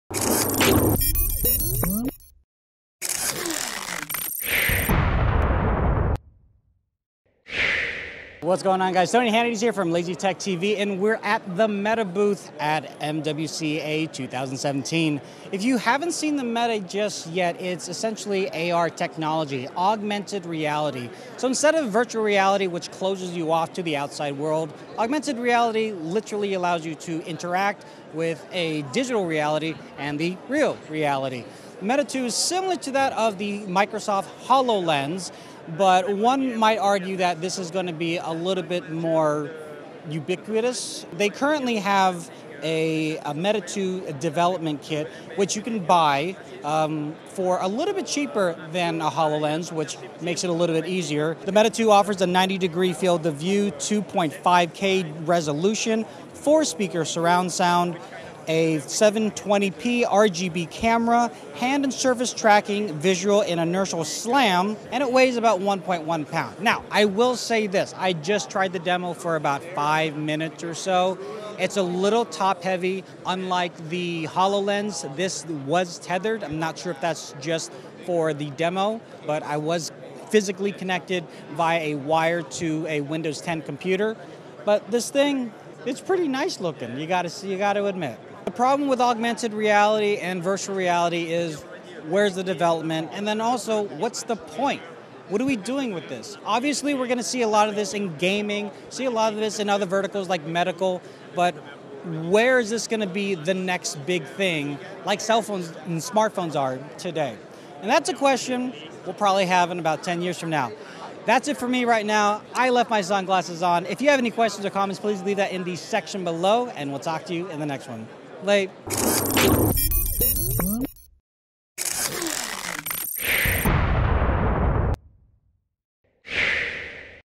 Checking Out the Meta 2 Augmented Reality Headset
Is AR better than VR? Well first we have to improve the technology and that's what the team at Meta want to do. But did they do it? Find out here!